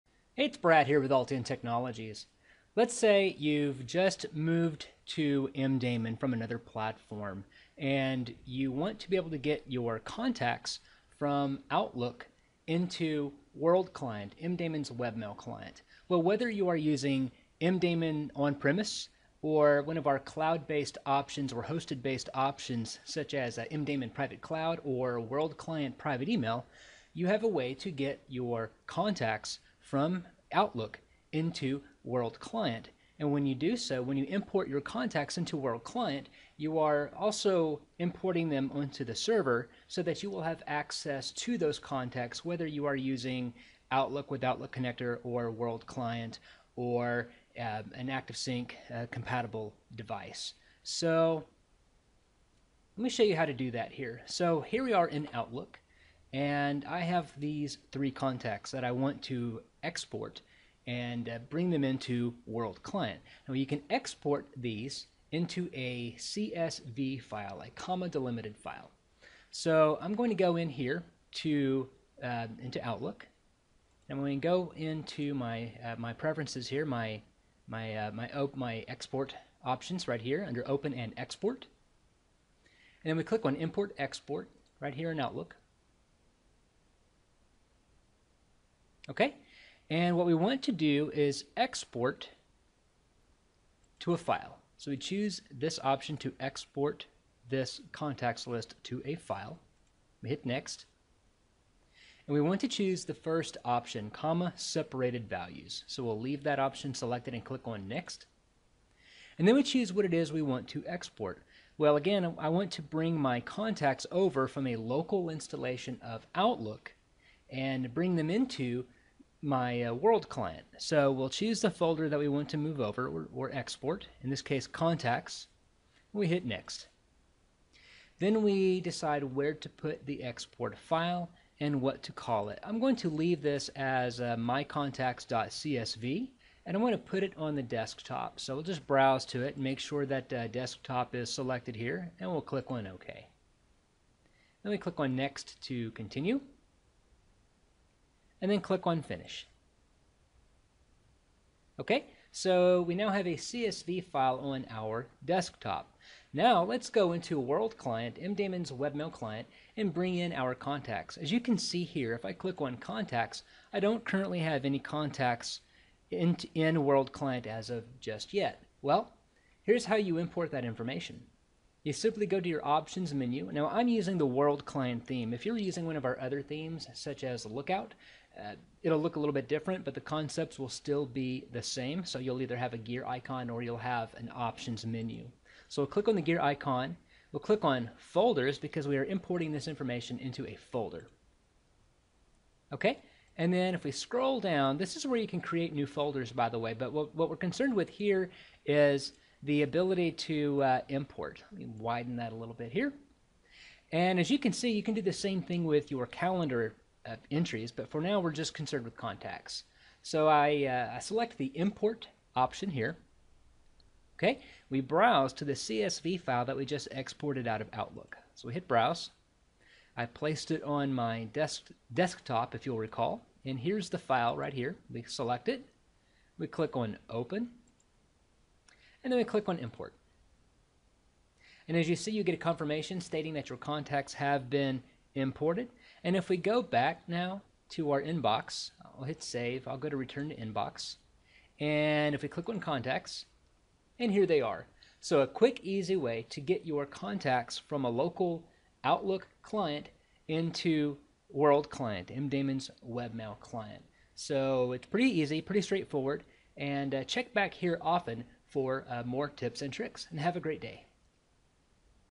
How to Transfer Contacts from Outlook to MDaemon Webmail
This tutorial video explains how to transfer your contacts and calendar entries from Microsoft Outlook or another email client to MDaemon Webmail. Simply export your contacts or calendar items to a CSV file, navigate to the Settings menu in MDaemon Webmail, click on Folders, select the Import button, and then upload your CSV file.

To learn more about MDaemon Webmail's security and collaboration features,